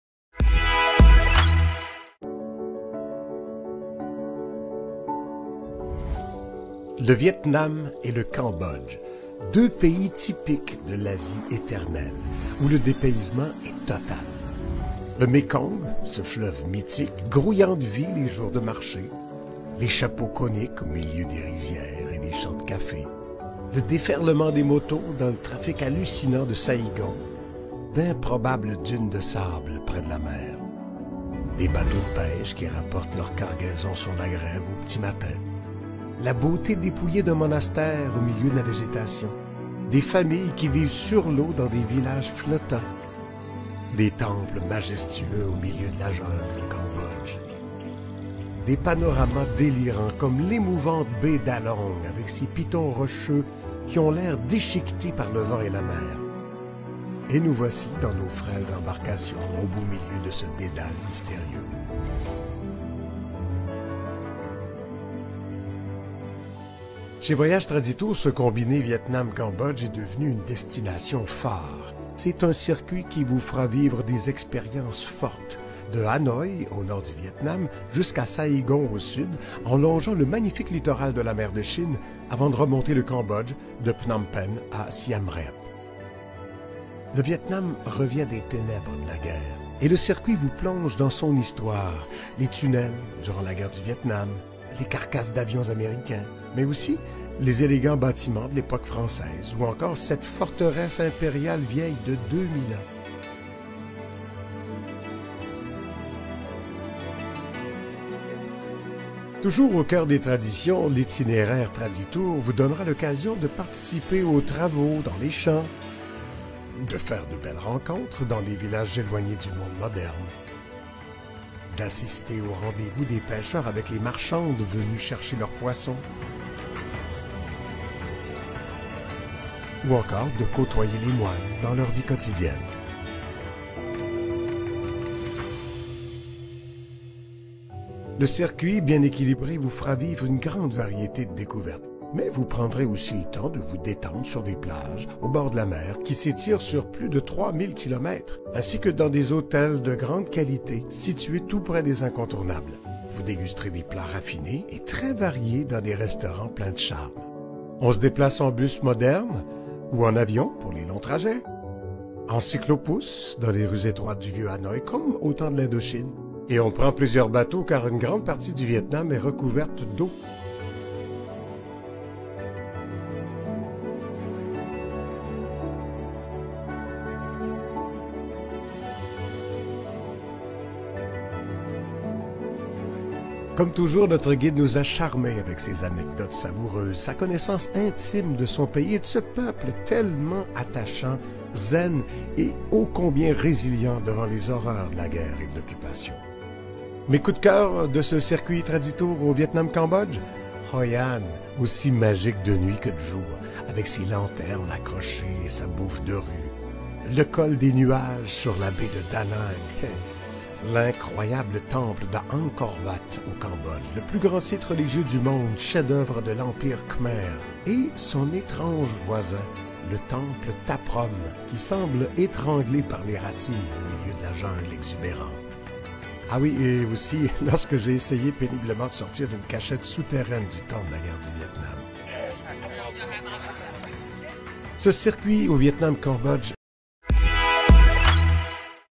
Voyage Vietnam: Bienvenue au Vietnam
Au Vietnam, le dépaysement est partout : en tuk-tuk dans le trafic étourdissant de Saigon encerclé par des centaines de scooters, dans les rizières où les dames avec leurs chapeaux coniques vous saluent ou lorsque vous êtes entouré de bateaux multicolores dans les marchés flottants au milieu du Mékong, le grand fleuve d’Asie. J’ai été charmé par les maisons coloniales de l’époque française, ébloui par la majesté de la citadelle de Hue, l’ancienne capitale, et subjugué par le grandiose temple d’Angkor Wat au Cambodge, le plus grand temple religieux du monde et son voisin le très intrigant temple Tha Prom étouffé par les racines de la jungle.